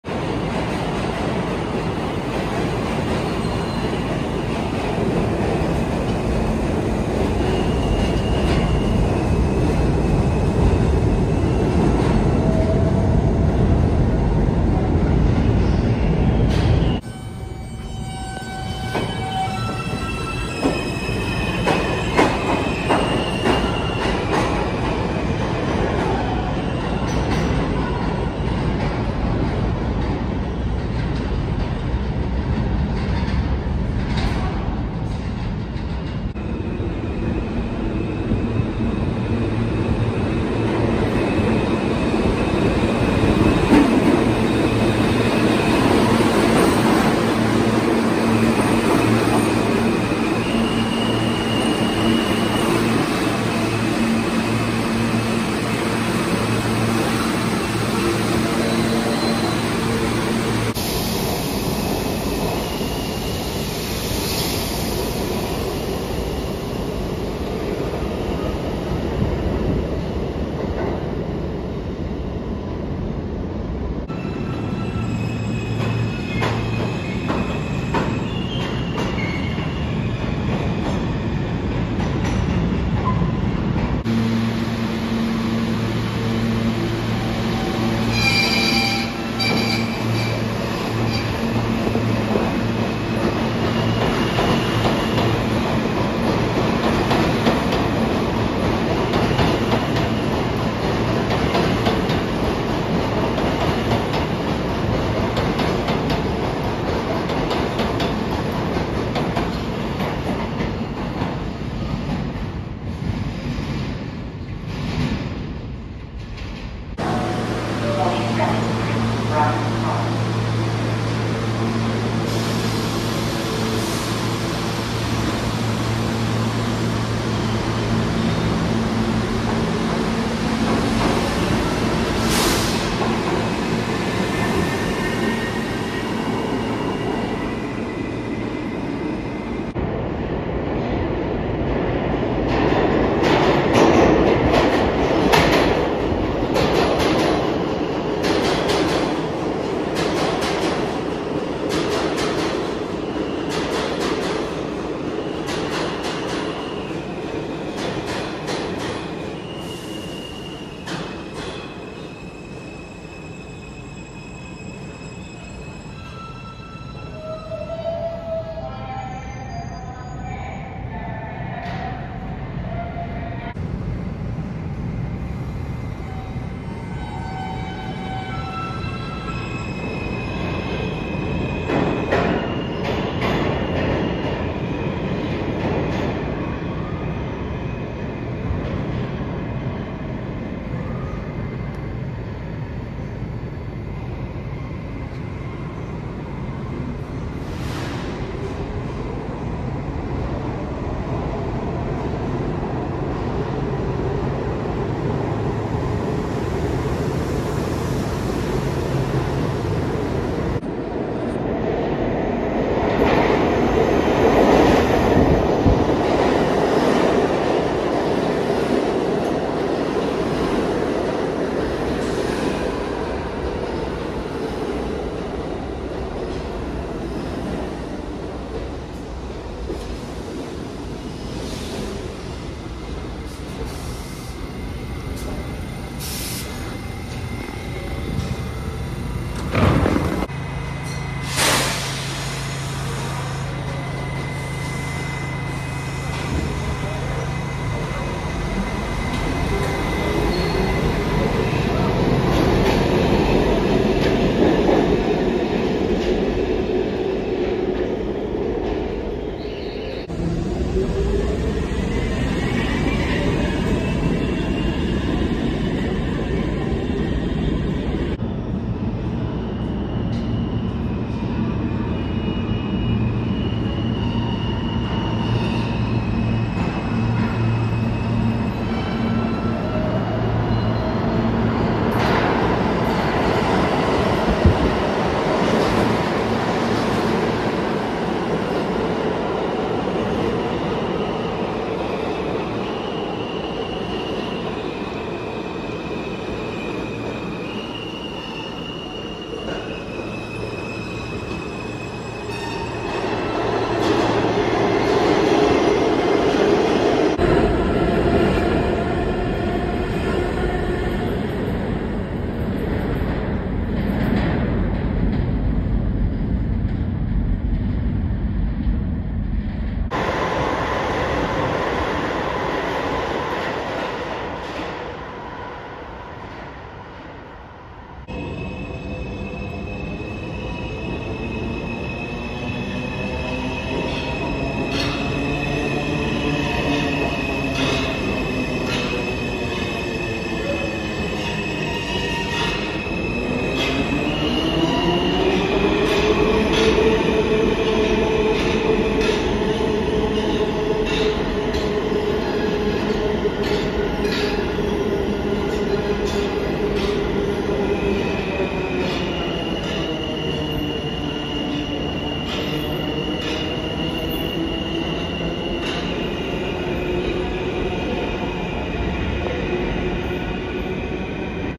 (B),(D),(F),(M) Train Action 2021
All videos recorded from September-December 2021 on an iPhone 13.
Train models:
(B): R68
(D): R68
(F): R160
(M): R160
Stations:
(B): 42nd St-Bryant Pk, 47-50 Sts-Rockefeller Ctr
(D): 47-50 Sts-Rockefeller Ctr, 59 St-Columbus Circle
(F): 42nd St Bryant Pk. 47-50 Sts-Rockefeller Ctr, Parsons Blvd, bypassing Woodhaven Blvd, 169 St (at the station and bypassing the station)
(M): 5th Av/53 St, Jackson Hts- Roosevelt Av, 47-50 Sts-Rockefeller Ctr, 63 Dr-Rego Park, Woodhaven Blvd